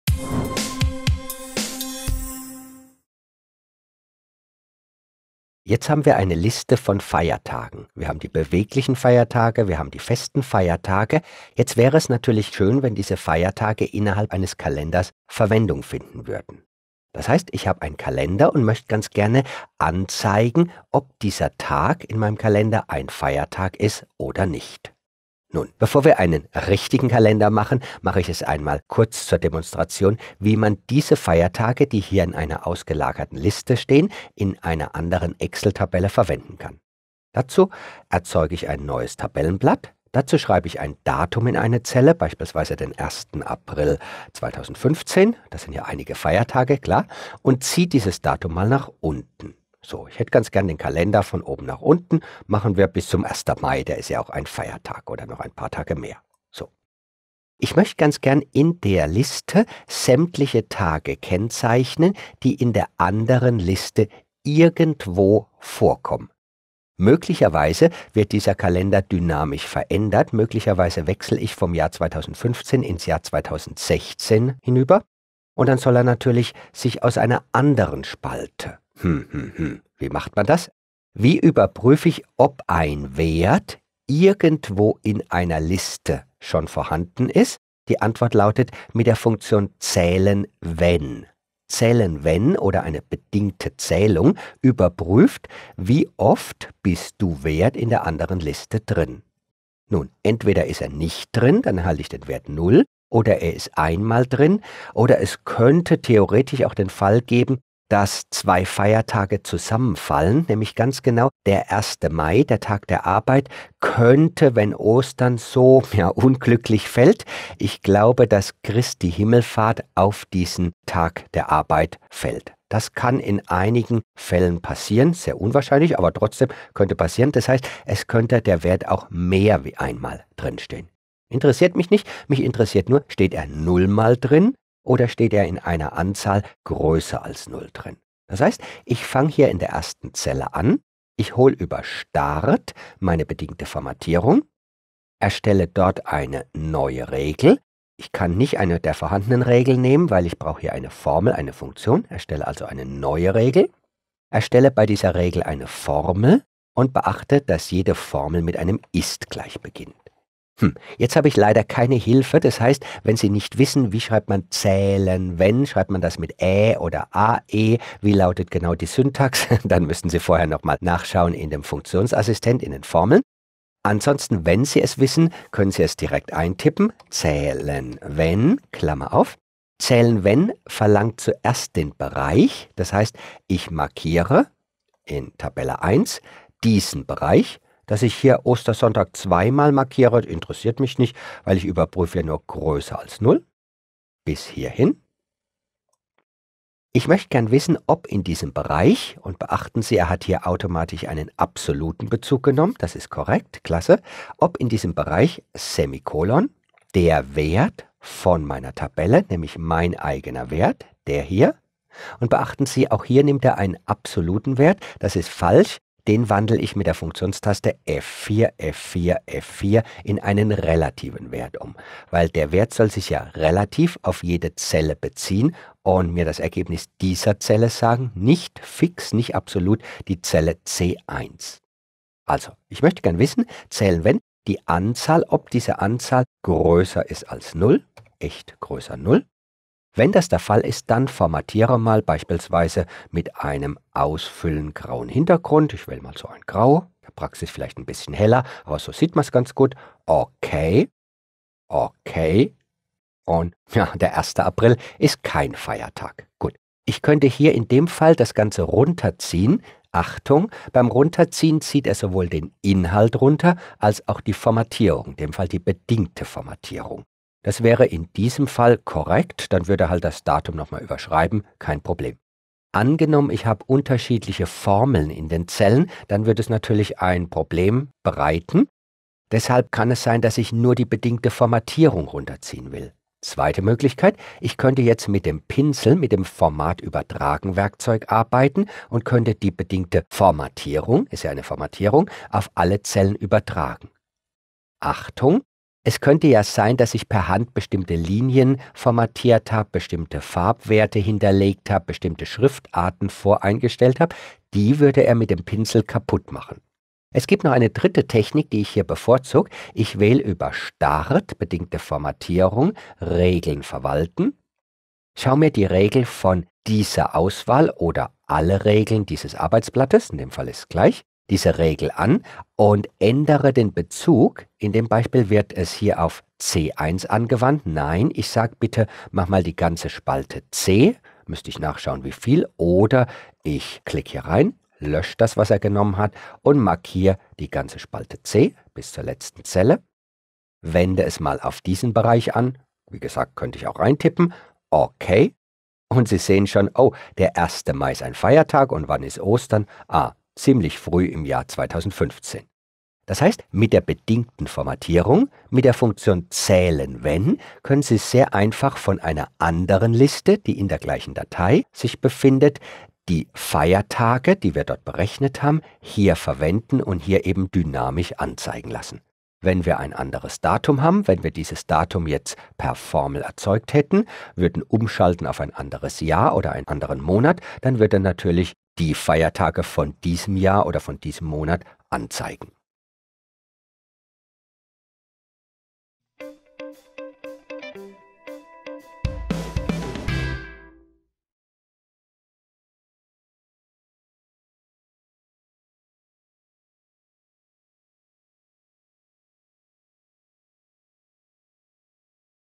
Kalender mit Excel erstellen Tutorial: Feiertage anzeigen (bedingte Formatierung) |video2brain.com
Komplettes Video-Training Sie möchten Feiertage in Ihrem Kalender gesondert hervorheben? Nutzen Sie dazu doch einfach die Excel-Funktion der bedingten Formatierung!
Dieses Tutorial ist ein Auszug aus Rene Martins neuem Video-Training „Excel-Workshop: Kalender erstellen. Dynamische Kalender für verschiedenste Zwecke.“